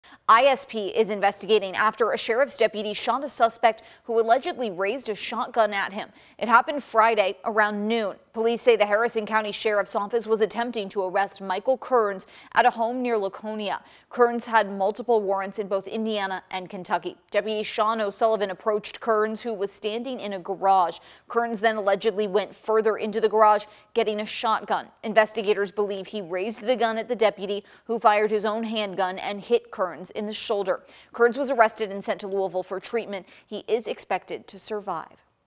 ISP investigating officer-involved shooting in Harrison County
Deputy Sean O'Sullivan went to the home attempting to arrest 28-year-old Michael W. Kerns, who has multiple warrants in both Indiana and Kentucky.